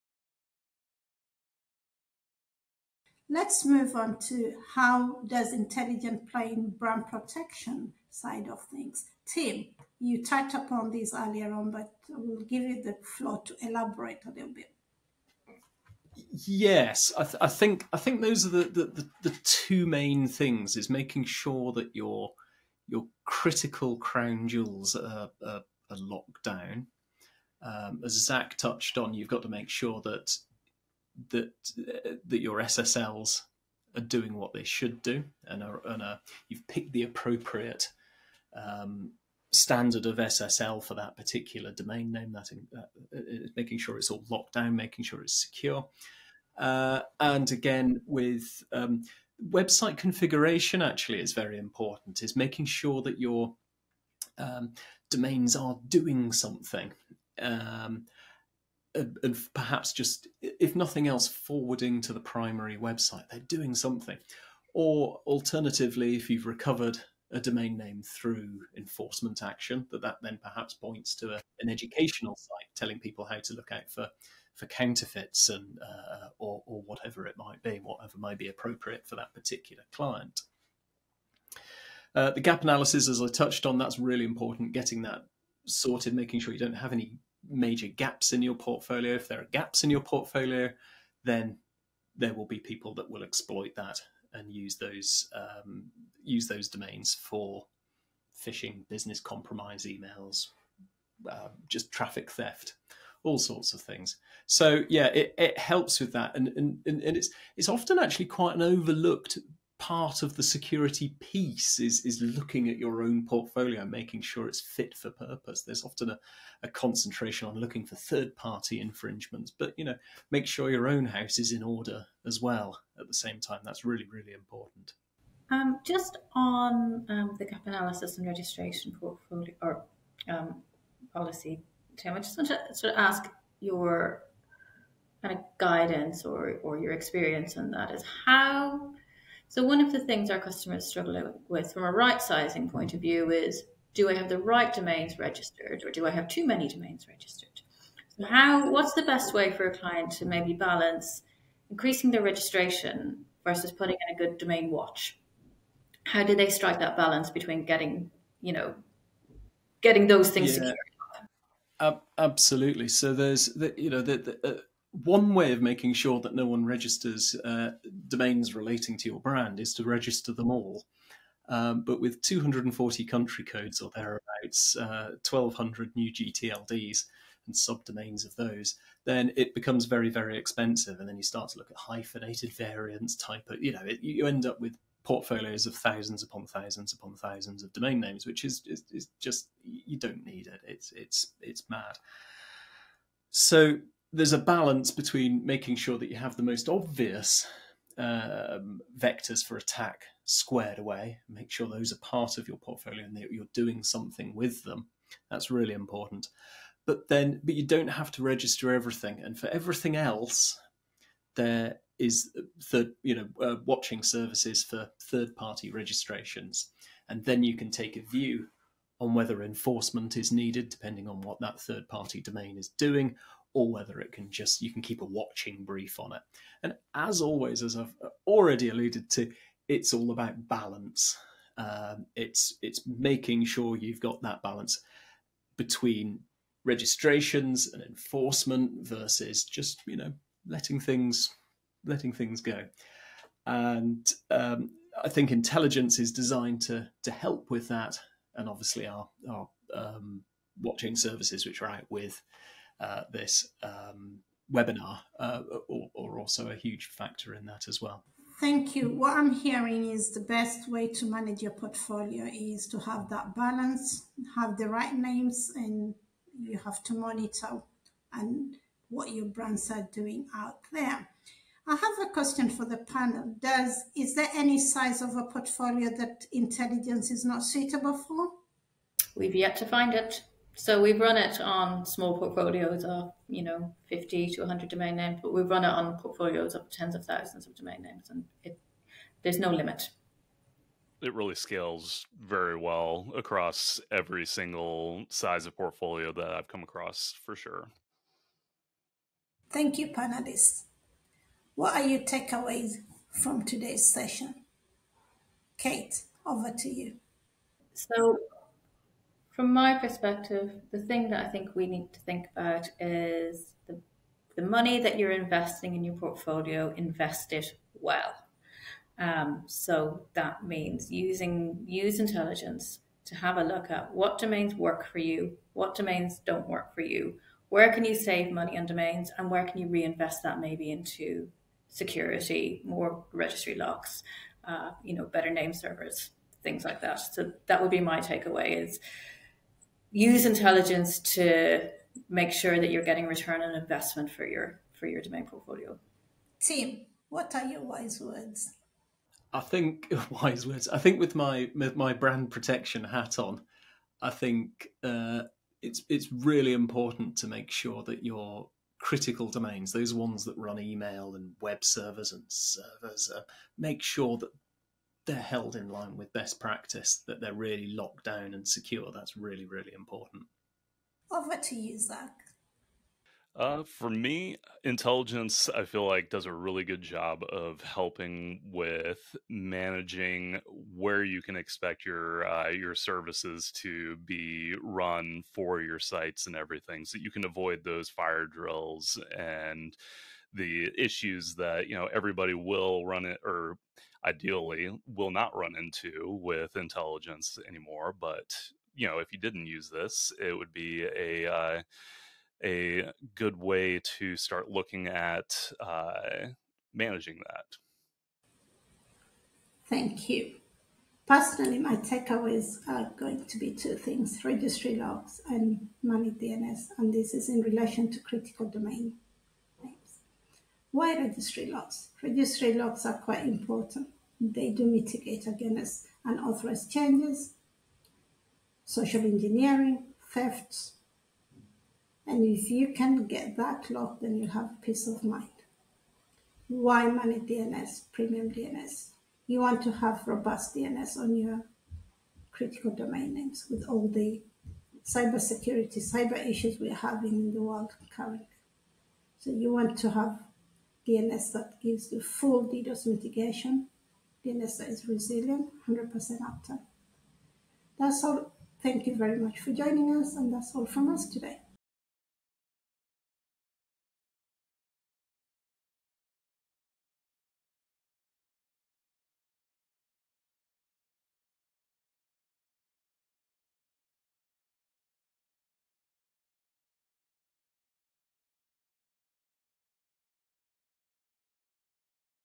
Com Laude Intelligence Webinar - how to ensure your brand is protected (part 3)
Watch our exclusive 3-part webinar and discover our revolutionary approach to domain portfolio management with Com Laude Intelligence.

In this third and final part we explore how Com Laude Intelligence can help you find balance in your portfolio, secure your brands, and ensure you have registered the right names.